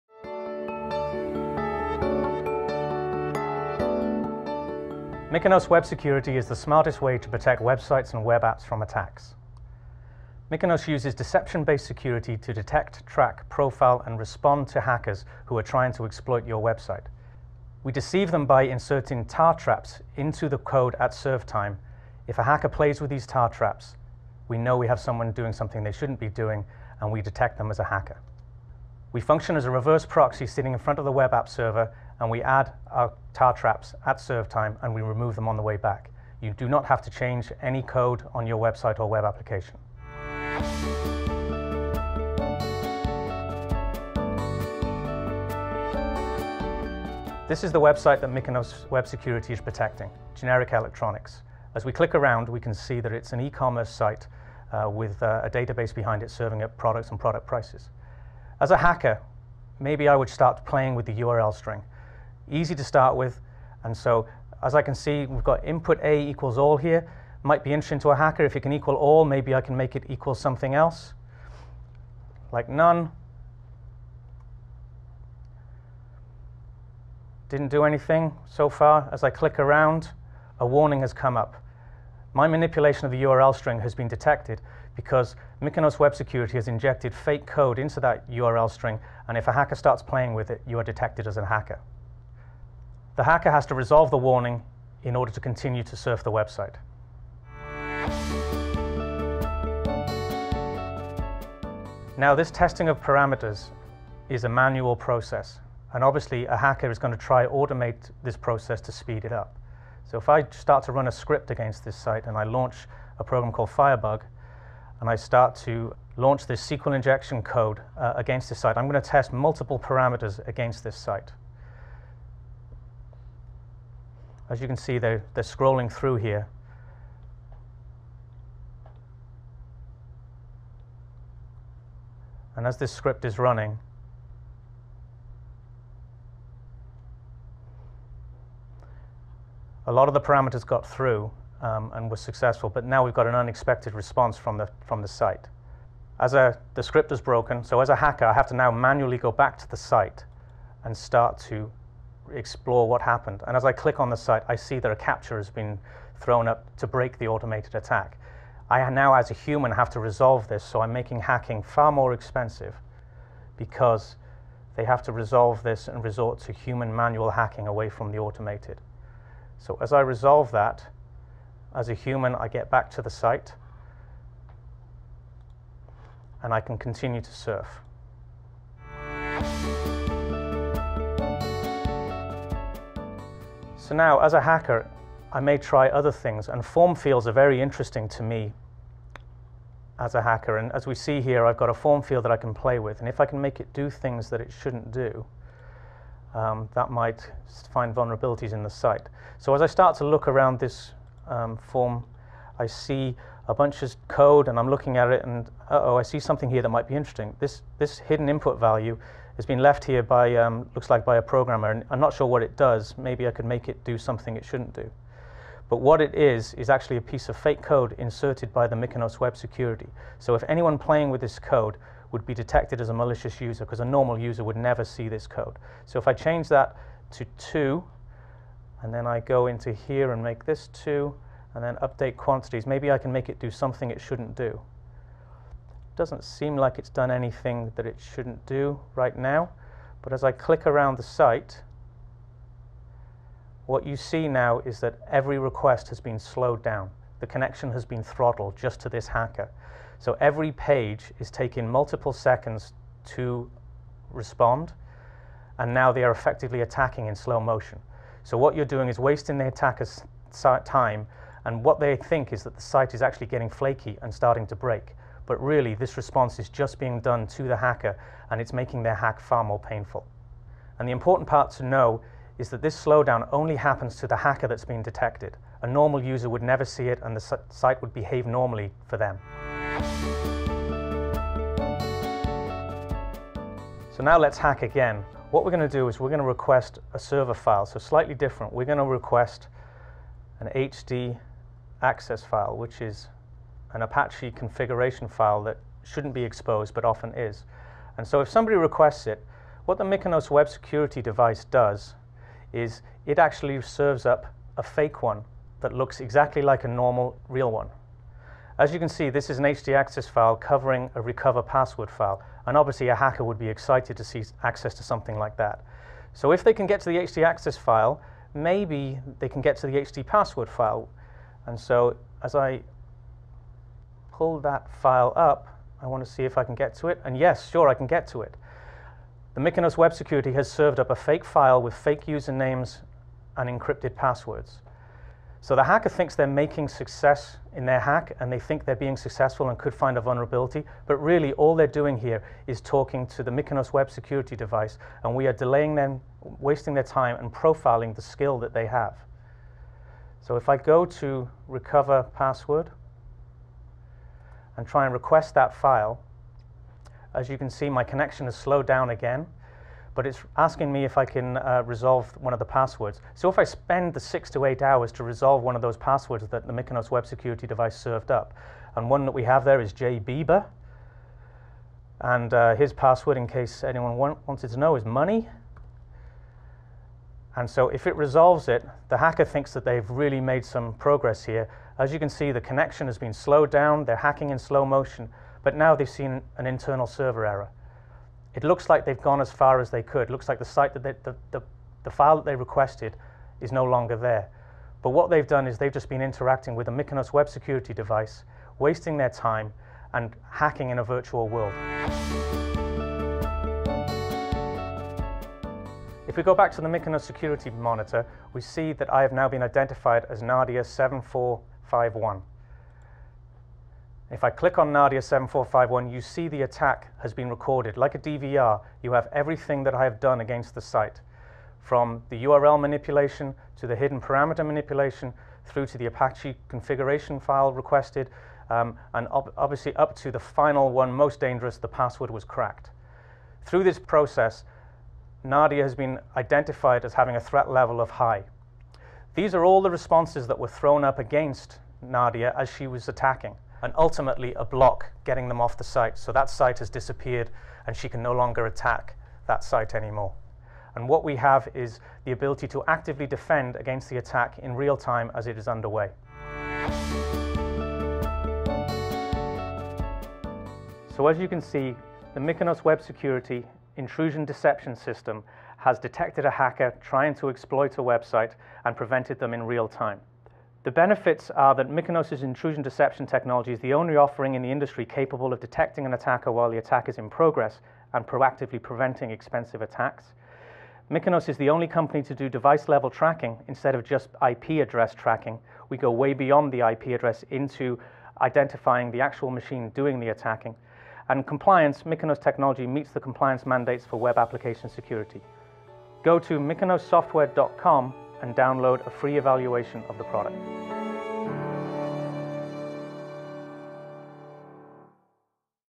An Inside Look: WebApp Secure (formerly Mykonos)
WebApp Secure (formerly Mykonos) is the smartest way to protect websites and web apps from attacks. WebApp Secure uses deception based security to detect, track, profile and respond to hackers who are trying to exploit your website. We function as a reverse proxy sitting in front of the web app server and we add tar traps at serve time and remove them on the way back. You do not have to change any code on your website or web application. We invite you to join us for a demonstration of WebApp Secure!